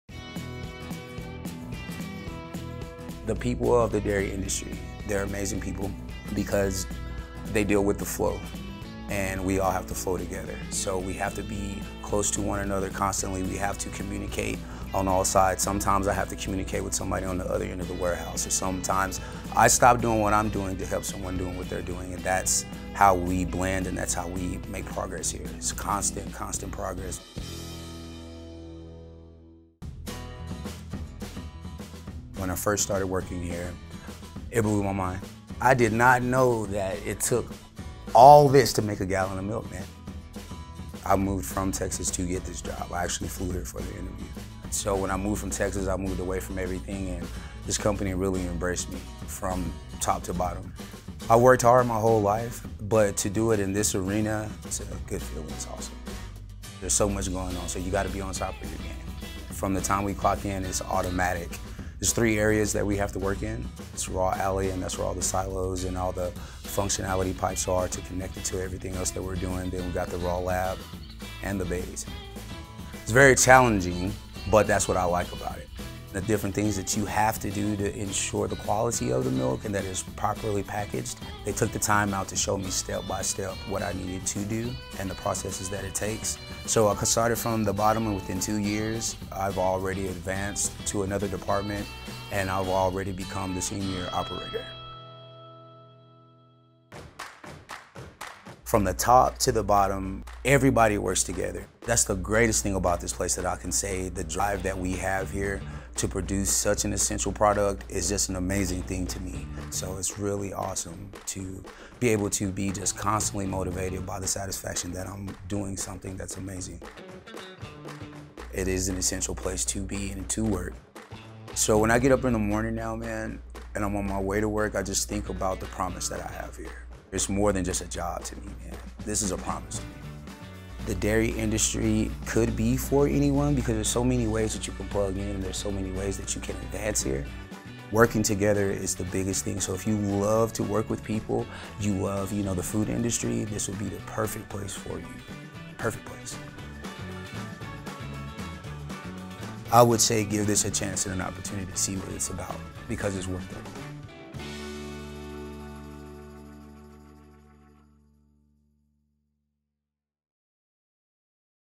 Perry's Ice Cream Careers in Dairy Production Operators presented by the IDFA
Hear from Production Operator Marcell Brazil on his experience in the dairy industry.